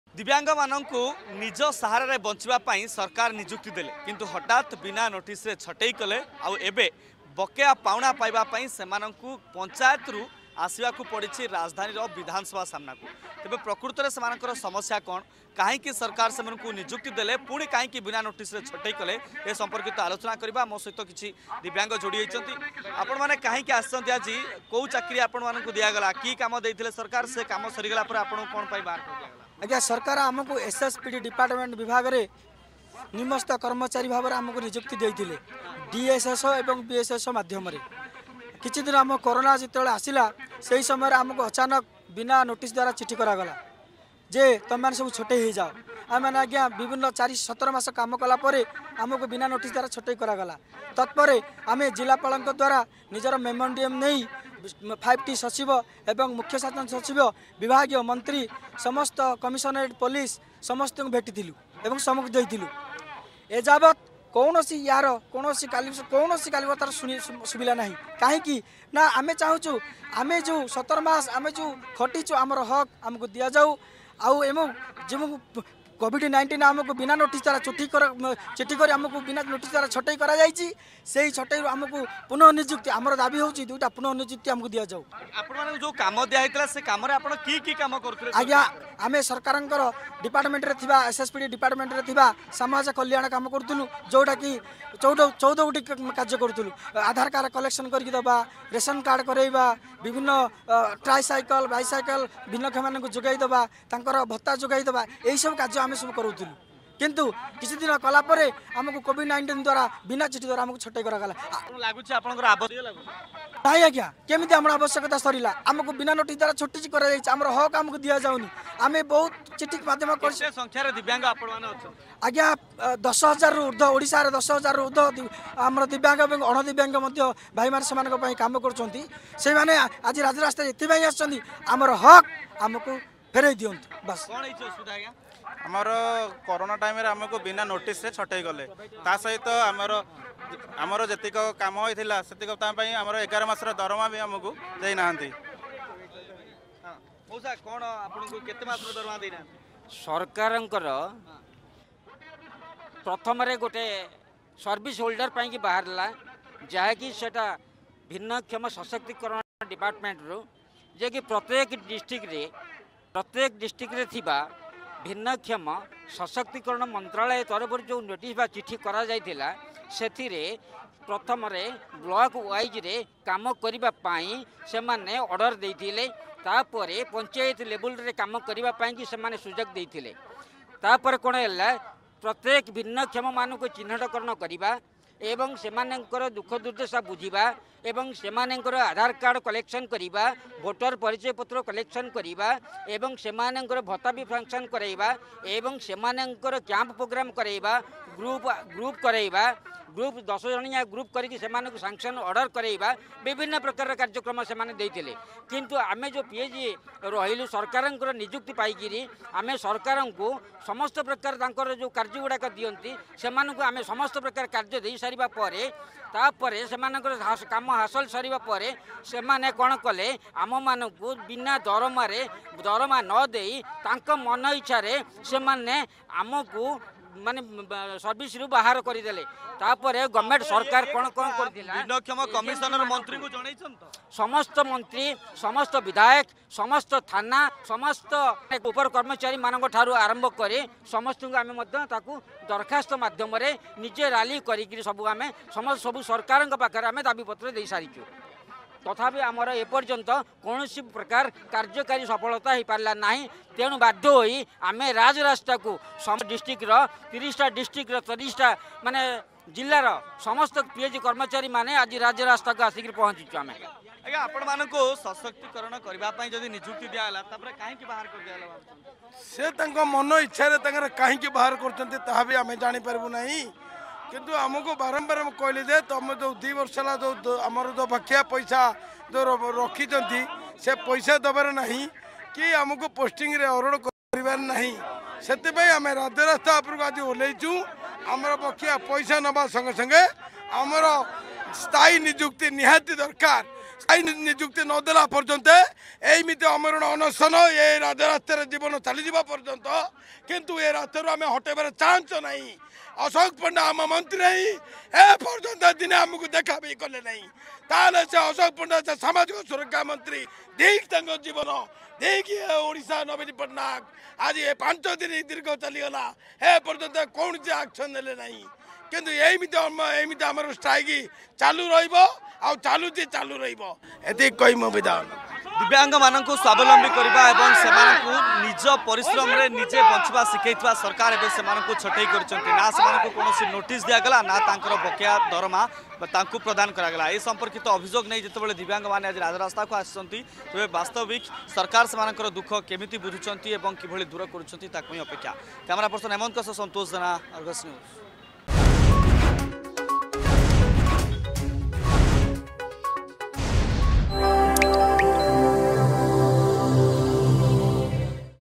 ରାଜ୍ୟ ସରକାର ବିନା ନୋଟିସରେ ଛଟେଇ କଲେ ଦିବ୍ୟାଙ୍ଗମାନଙ୍କୁ
ବିନା ନୋଟିସରେ ଦିବ୍ୟାଙ୍ଗମାନଙ୍କୁ ଛଟେଇ କଲେ ରାଜ୍ୟ ସରକାର
ଶୁଣନ୍ତୁ କଣ ରହିଛି ଦିବ୍ୟାଙ୍ଗମାନଙ୍କ ପ୍ରତିକ୍ରିୟା...